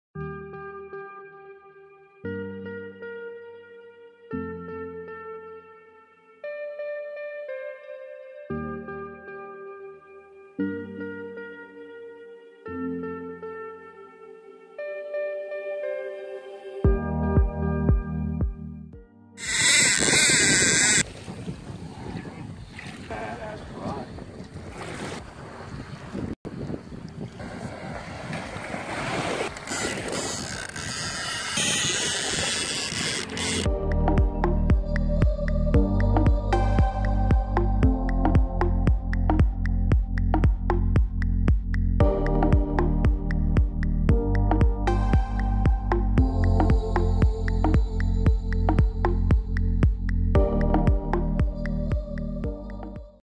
Monster Stingray
BCBL's Capt.Cguillen fighting a huge Southern Stingray.